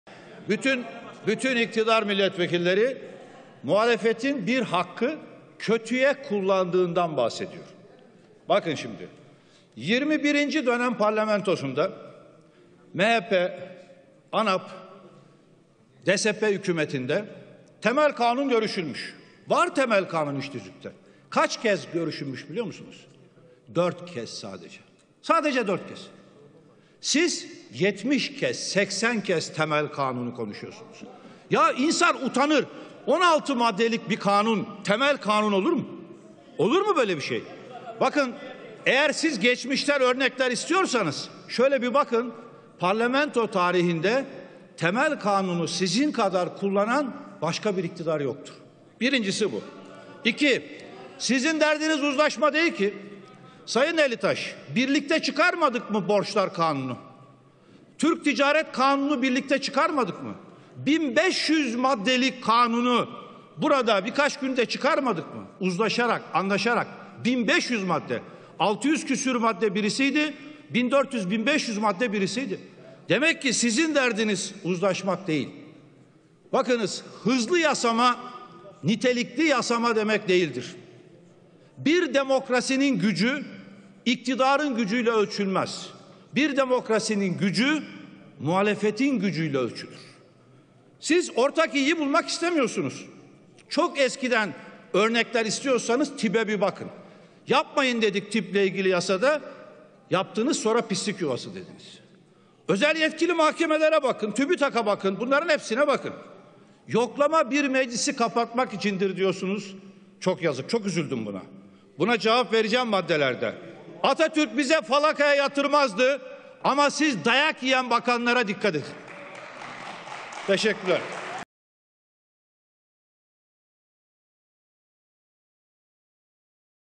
Hızlı Yasama Nitelikli Yasama Demek Değildir!
Yalova Milletvekilimiz Muharrem İnce: Parlamento tarihinde temel kanunu sizin kadar kullanan başka bir iktidar yoktur. Sizin derdiniz uzlaşma değil! Bir demokrasinin gücü iktidarın gücüyle ölçülmez, bir demokrasinin gücü muhalefetin gücüyle ölçülür. Siz, ortak iyiyi bulmak istemiyorsunuz!